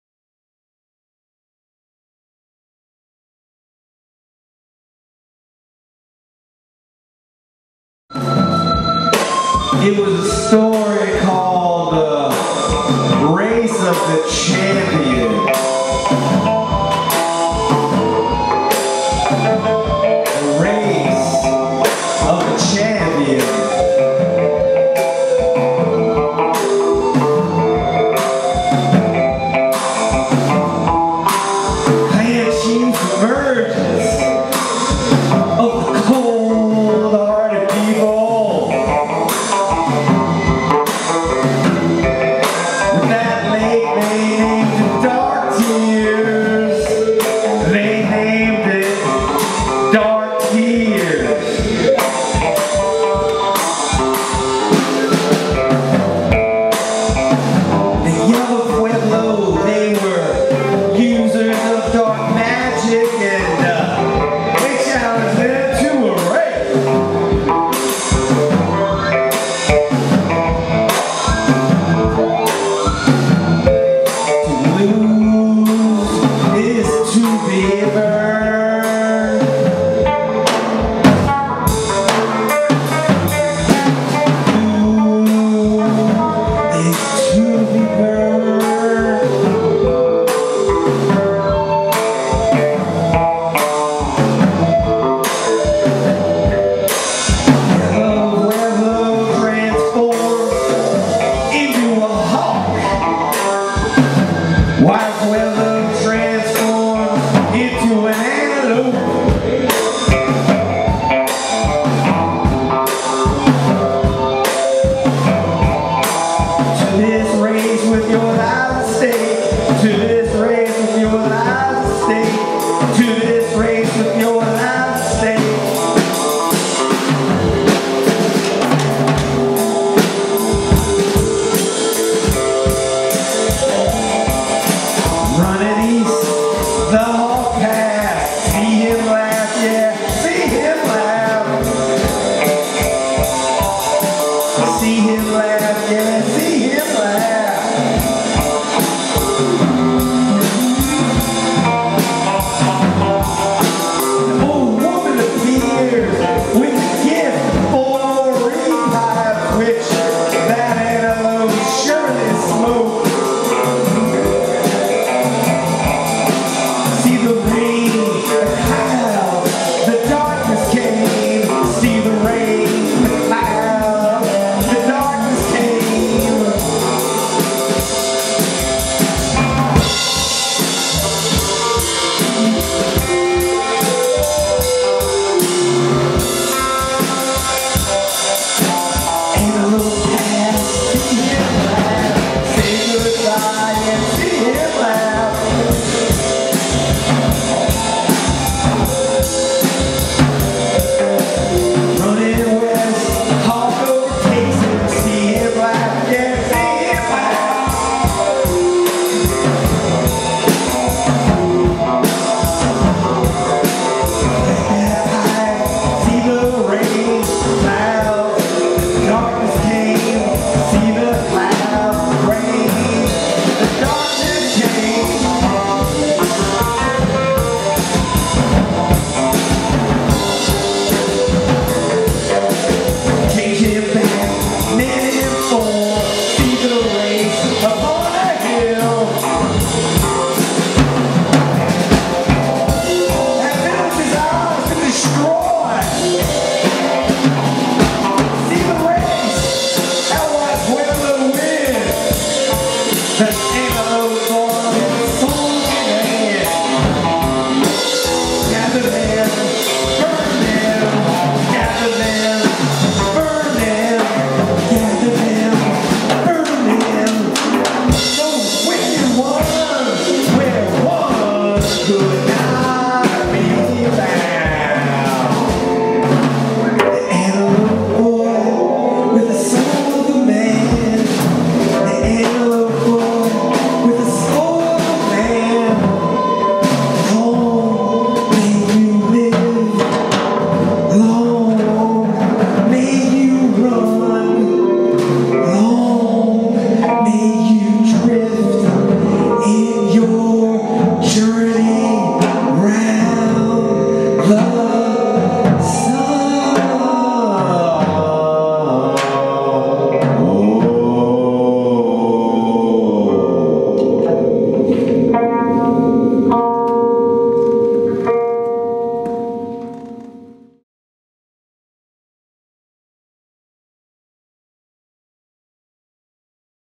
James Harrar's CINEMA SOLORIENS with DAEVID ALLEN
CiINEMA SOLORIENS are:
James Harrar-voice & words
Daevid Allen-guitar & effects
Marshall Allen-casiotone
Rogier Smal-drums & percussion

live 2013